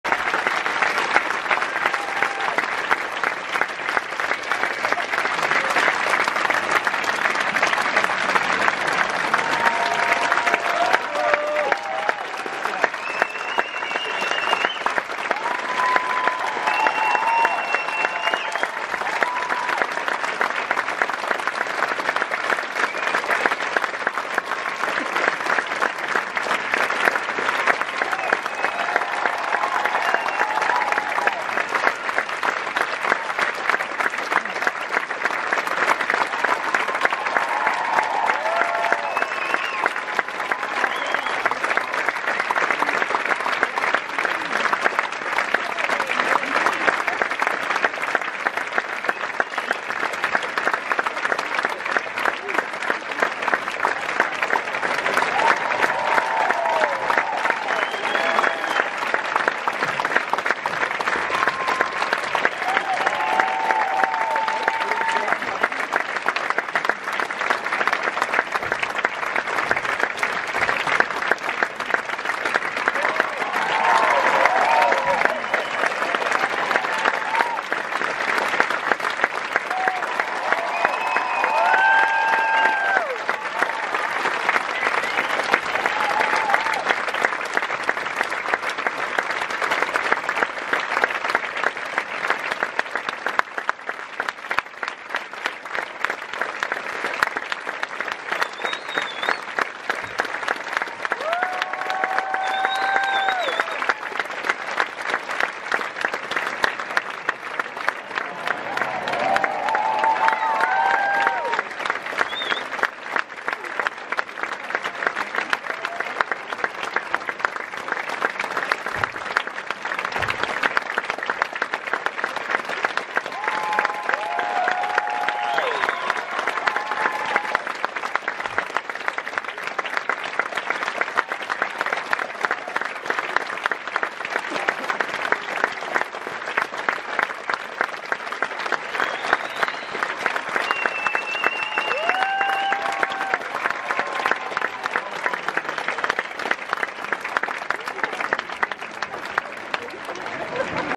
War Horse Curtain Call - The Lowry - Sunday 22nd September
At the heart of War Horse lies the extraordinary bond between young Albert Narracott and his beloved horse, Joey. Set against the backdrop of World War I, Albert’s life is upended when his father sells Joey to the cavalry, sending the horse to the front lines of war-torn France. Desperate to be reunited with his trusted companion, Albert, too young to enlist, embarks on a perilous and deeply personal journey through the horrors of battle.